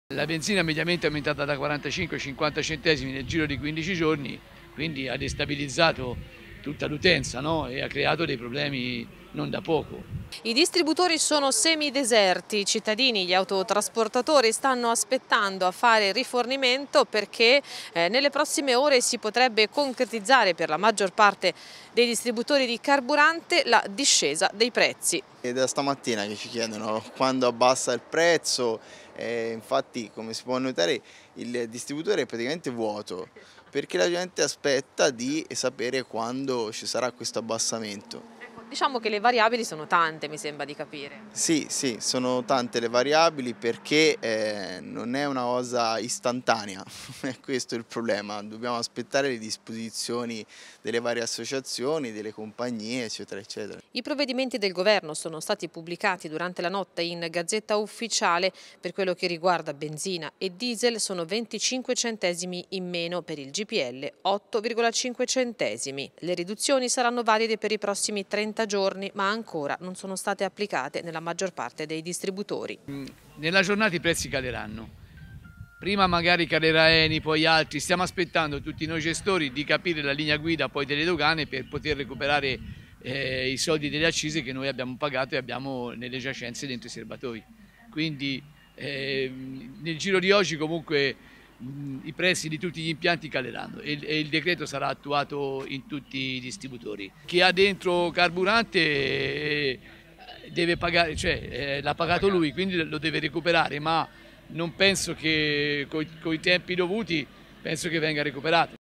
Arezzo, i prezzi dei carburanti verso il ribasso
Il prezzo del carburante è in discesa, anche se ancora alcuni distributori mantengono prezzi elevati. I motivi sono vari: dalle politiche aziendali allo smercio ma una cosa è quasi certa il prezzo di benzina e gasolio continuerà a scendere